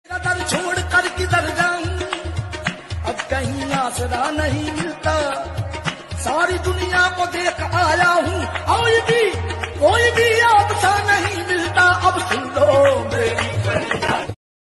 Hazrat Shahe Alam Sarkar Dargah Sharif New Status | Ahmedabad, Gujarat
This is a new video with some holy glimpses of Dargah Hazrat Shah e Alam Sarkar situated at Ahmedabad (Gujarat). This short has some inside and outside view of Hazrat Shahe Alam Sarkar Dargah Sharif.

The full name of Shahe Alam Sarkar is Hazrat Syed Shabeeh e Mustafa Muhammad Sirajuddin Al-Ma'roof Shah e Alam Soharwardi Ahmedabadi (RadiAllahu Anhu). His holy Shrine is located at the city of Ahmedabad in Gujarat.
He is a great sufi saint of the Silsila e Soharward.